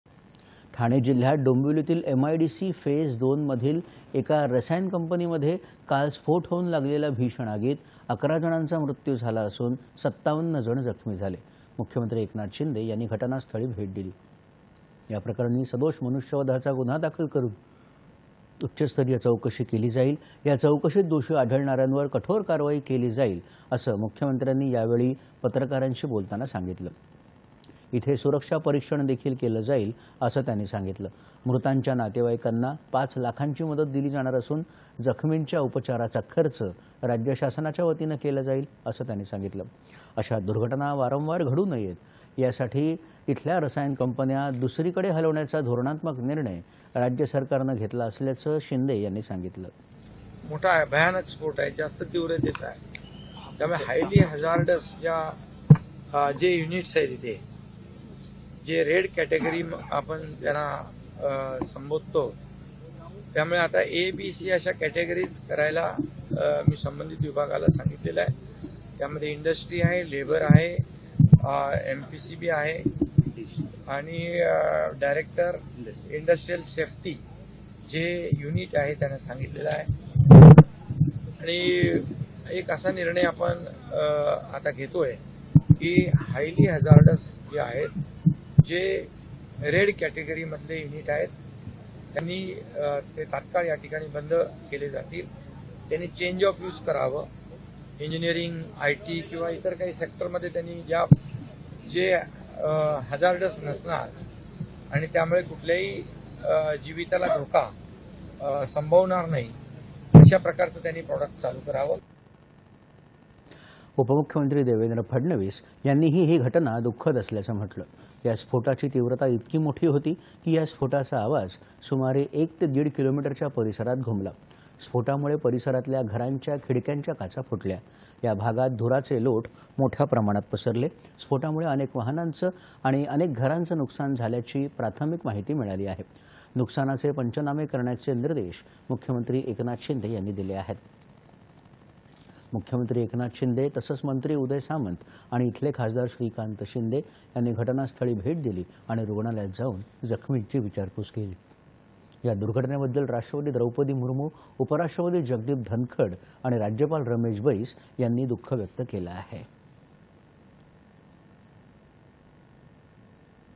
डोंबिवली एमआयडीसीमध्ये झालेल्या स्फोटात अकरा जणांचा मृत्यू | DD Sahyadri News
ठाणे जिल्हयात डोंबिवलीतल्या एमआयडीसी फेज दोन मधल्या एका रसायन कंपनीमध्ये काल स्फोट होऊन लागलेल्या भीषण आगीत ११ जणांचा मृत्यू झाला असून ५७ जण जखमी झाले. मुख्यमंत्री एकनाथ शिंदे यांनी घटनास्थळी भेट दिली. याप्रकरणी सदोष मनुष्यवधाचा गुन्हा दाखल करून उच्च स्तरीय चौकशी केली जाईल, या चौकशीत दोषी आढळणाऱ्यांवर कठोर कारवाई केली जाईल असं मुख्यमंत्र्यांनी यावेळी पत्रकारांशी बोलताना सांगितलं. इथे सुरक्षा परीक्षण देखील केलं जाईल असं त्यांनी सांगितलं. मृतांच्या नातेवाईकांना पाच लाखांची मदत दिली जाणार असून जखमींच्या  उपचाराचा खर्च राज्य शासनाच्या वतीनं केला जाईल, असं त्यांनी सांगितलं. अशा दुर्घटना वारंवार घडू नयेत म्हणून, इथल्या रसायन कंपन्या दुसरीकडे हलवण्याचा धोरणात्मक निर्णय राज्य सरकारनं घेतला असल्याचं शिंदे यांनी सांगितलं.

---होल्ड--सिंक-- एकनाथ शिंदे, मुख्यमंत्री.

उपमुख्यमंत्री देवेंद्र फडणवीस यांनीही ही घटना दुःखद असल्याचं म्हटलं. या स्फोटाची तीव्रता इतकी मोठी होती की, या स्फोटाचा आवाज सुमारे एक ते दीड किलो मीटरच्या परिसरात घुमला. स्फोटामुळे परिसरातल्या घरांच्या खिडक्यांच्या काचा फुटल्या. या भागात धुराचे लोट मोठया प्रमाणात पसरले. स्फोटामुळे अनेक वाहनांचं आणि अनेक घरांचं  नुकसान झाल्याची प्राथमिक माहिती मिळाली आहे. नुकसानाचे पंचनामे करण्याचे निर्देश, मुख्यमंत्री एकनाथ शिंदे यांनी दिले आहेत. मुख्यमंत्री एकनाथ शिंदे तसंच मंत्री उदय सामंत आणि इथले खासदार श्रीकांत शिंदे यांनी घटनास्थळी भेट दिली आणि रुग्णालयात जाऊन जखमींची विचारपूस केली. या दुर्घटनेबद्दल राष्ट्रपती द्रौपदी मुर्मू , उपराष्ट्रपती जगदीप धनखड आणि राज्यपाल रमेश बैस यांनी दुःख व्यक केलं आहे.

8.30am | 11.00am | 1.00pm | 4.30pm | 7.00pm | 9.30pm